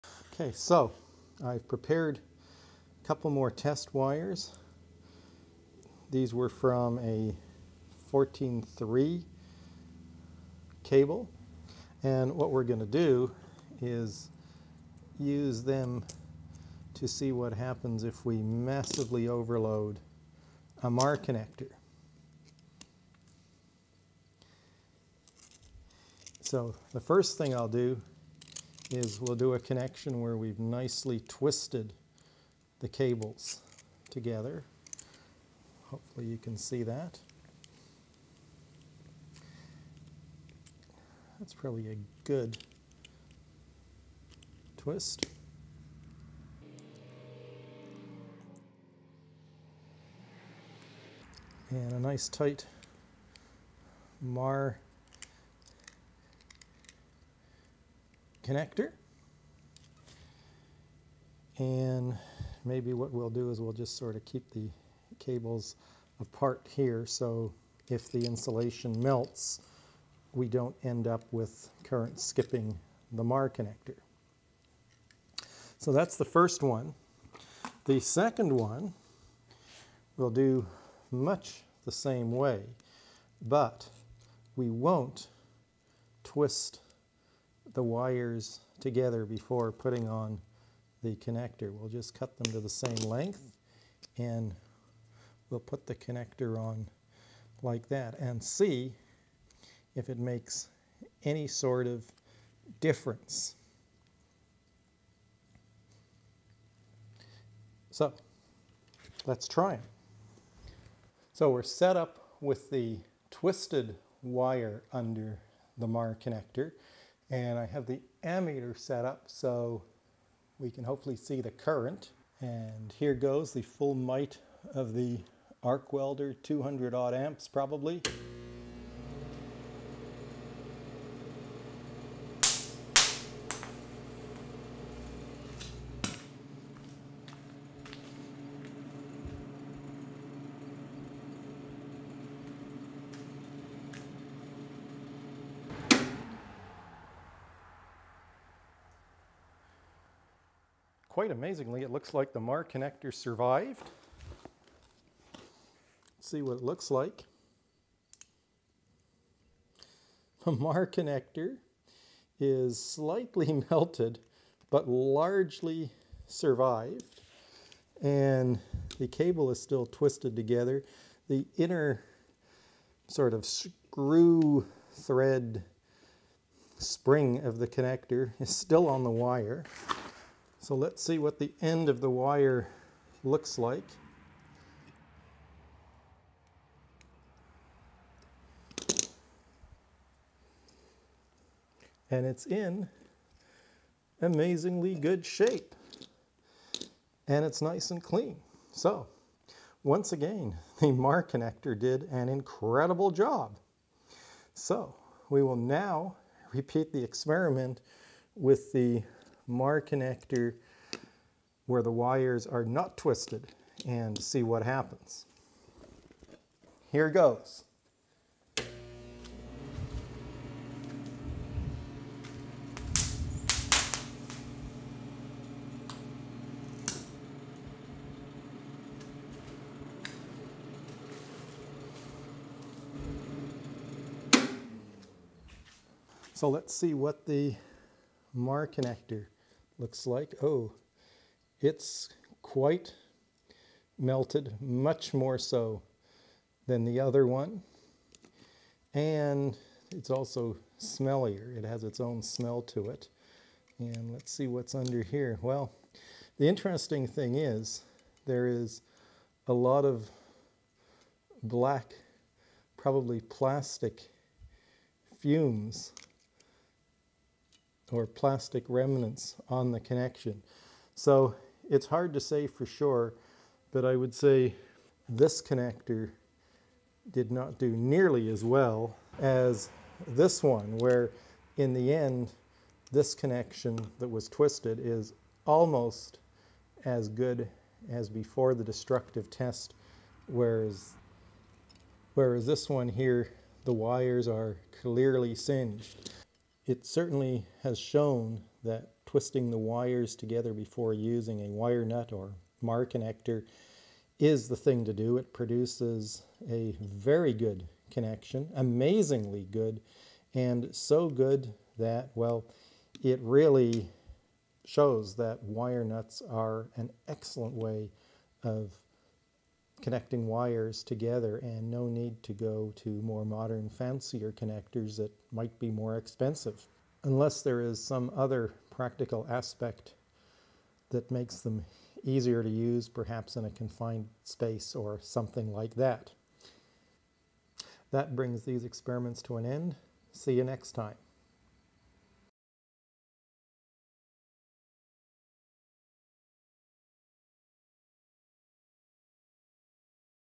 270 Amps through Wire-Nuts aka Marr Connectors
Here we test "good old fashioned" Wire-Nuts or Marr connectors to see if twisting the 14/2 wires together before screwing on the connector makes any difference.  I was amazed how well such a connection can survive a massive overload - in this case 270 Amps compared to the 15 or 20 Amps  a connector of that sized would normally be expected to handle. Used properly, it turns these connectors are more robust than the wires attached to them.